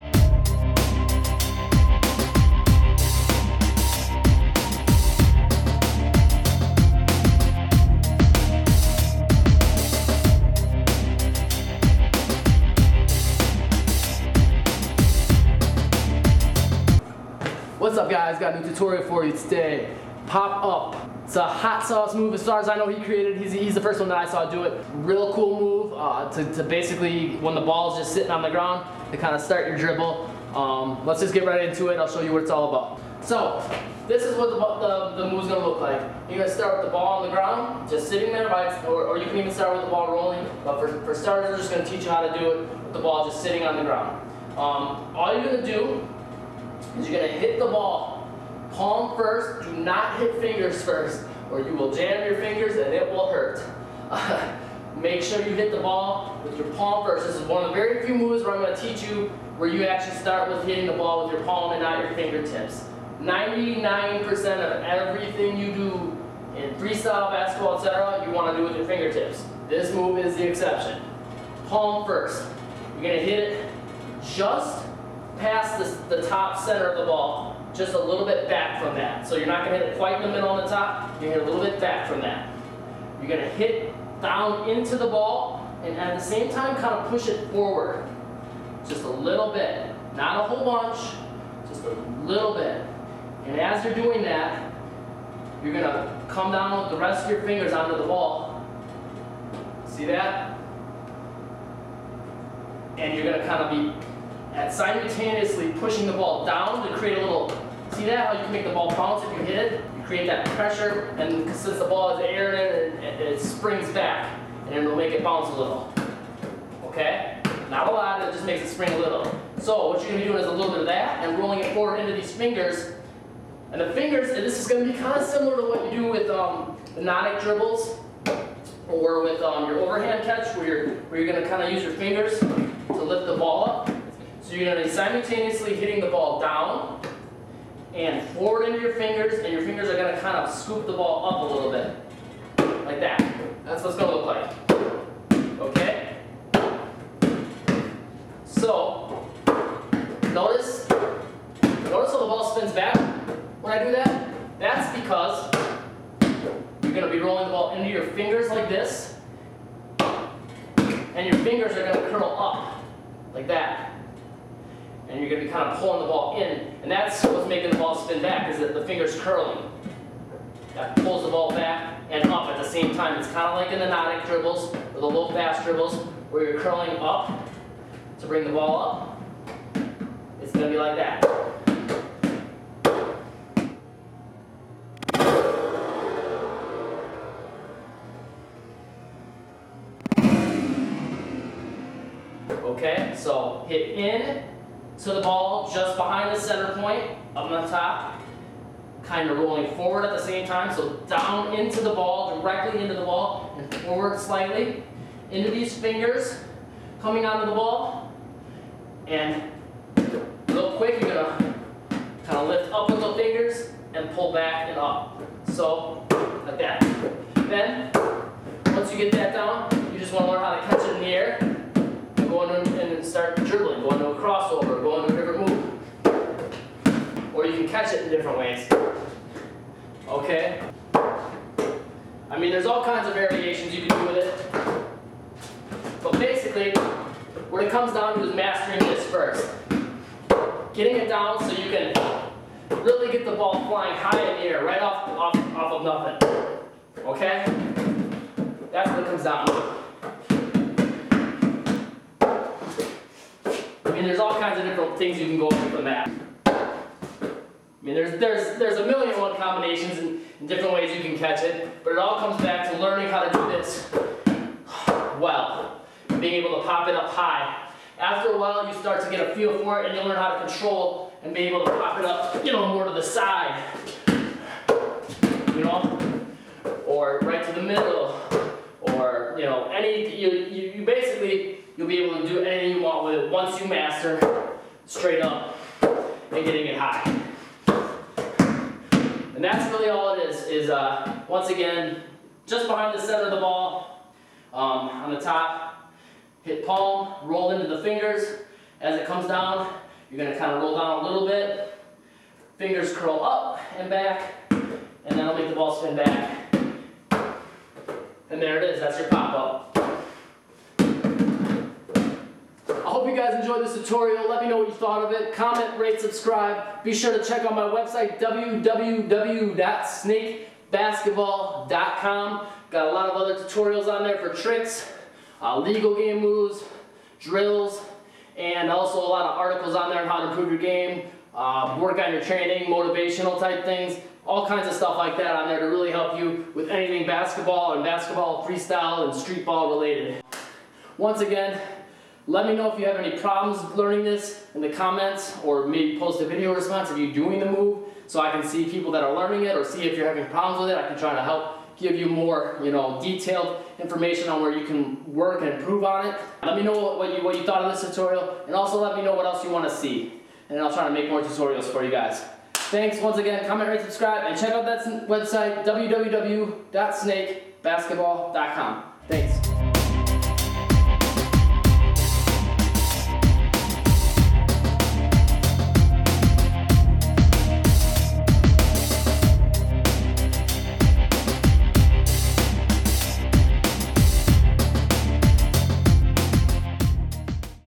Learn Basketball Tricks: Pop Up Tutorial | Snake
If you want to learn basketball tricks, the Pop Up is good one to start with.  Snake demonstrates how to do the Pop Up step by step so you can learn how to do this cool basketball trick quickly.